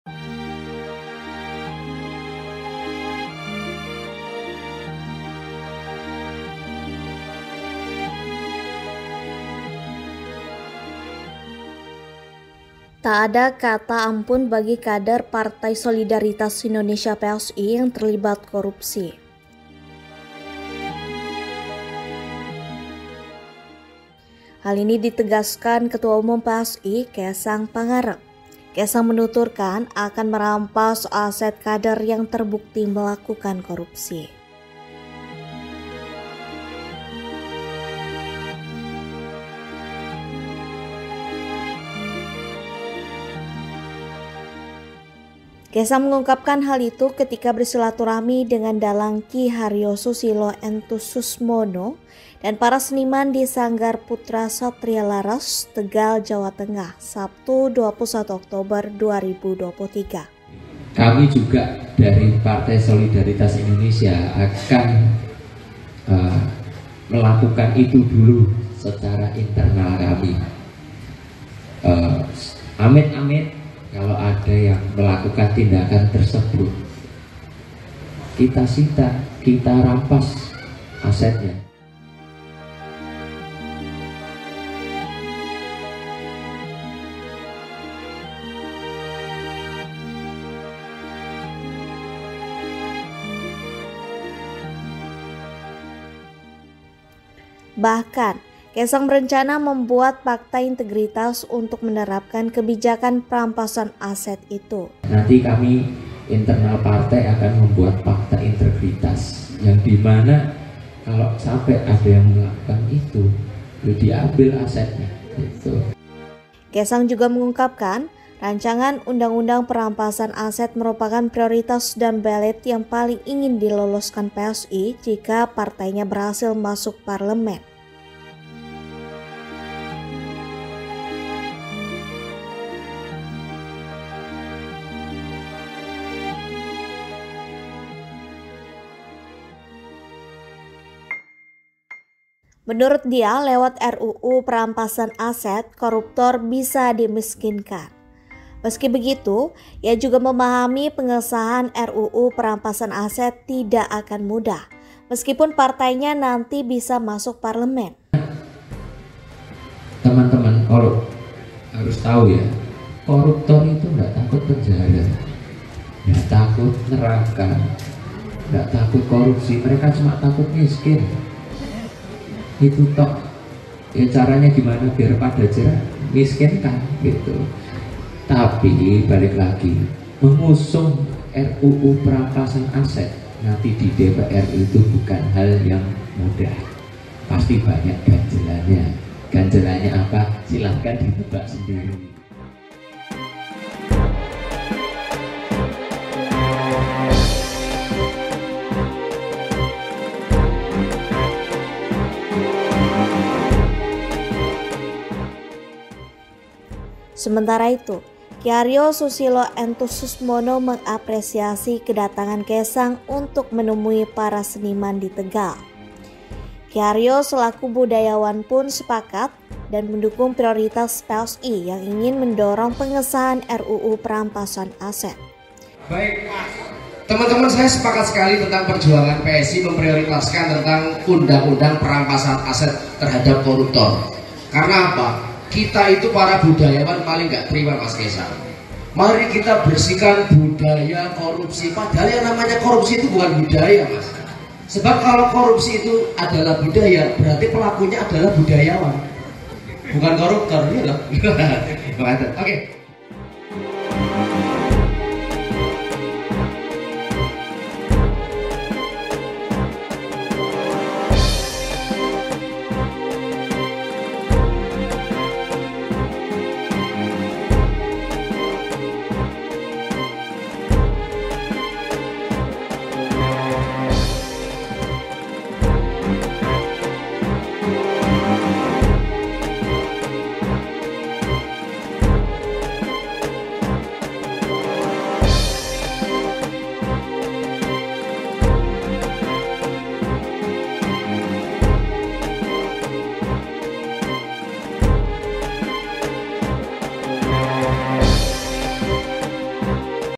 Tak Ada Ampun, Kaesang akan Miskinkan Kader PSI yang Terlibat Korupsi, Pakai Pakta Integritas
Tak ada kata ampun bagi kader Partai Solidaritas Indonesia (PSI) yang terlibat korupsi.

Hal ini ditegaskan Ketua Umum PSI, Kaesang Pangarep.

Kaesang menuturkan, akan merampas aset kader yang terbukti melakukan korupsi.

Kaesang mengungkapkan hal itu ketika bersilaturahmi dengan dalang Ki Haryo Susilo Enthus Susmono dan para seniman di Sanggar Putra Satria Laras, Tegal, Jawa Tengah, Sabtu (21/10/2023).

"Kami juga dari PSI akan melakukan itu (perampasan aset) dulu secara internal kami.

Amit-amit kalau ada yang melakukan tindakan (korupsi) tersebut, kami sita, kami rampas asetnya," kata Kaesang.

Bahkan, Kaesang berencana membuat pakta integritas untuk menerapkan kebijakan perampasan aset itu.

"Nanti kami internal partai akan membuat pakta integritas yang di mana kalau sampai ada yang melakukan itu, ya diambil asetnya," ujar dia.

Kaesang juga mengungkapkan, Rancangan Undang-Undang Perampasan Aset merupakan prioritas dan beleid yang paling ingin diloloskan PSI jika partainya berhasil masuk parlemen.

Menurut dia, lewat RUU Perampasan Aset, koruptor bisa dimiskinkan.

Meski begitu, ia juga memahami pengesahan RUU Perampasan Aset tidak akan mudah, meskipun partainya nanti bisa masuk parlemen.

"Soalnya teman-teman, harus tahu ya, koruptor itu enggak takut penjara. Enggak takut neraka, enggak takut korupsi, mereka cuma takut miskin.

Itu tok. Caranya gimana biar pada jera? Miskinkan," tutur Kaesang.

Sementara itu, Ki Haryo Susilo Enthus Susmono mengapresiasi kedatangan Kaesang untuk menemui para seniman di Tegal.

Ki Haryo selaku budayawan pun sepakat dan mendukung prioritas PSI yang ingin mendorong pengesahan RUU Perampasan Aset.

"Para budayawan paling enggak terima (adanya korupsi), Mas Kaesang.

Maunya kami bersihkan budaya korupsi, padahal yang namanya korupsi itu bukan budaya, Mas.

Sebab kalau korupsi itu budaya, berarti pelakunya adalah budayawan, bukan koruptor," ujar Ki Haryo.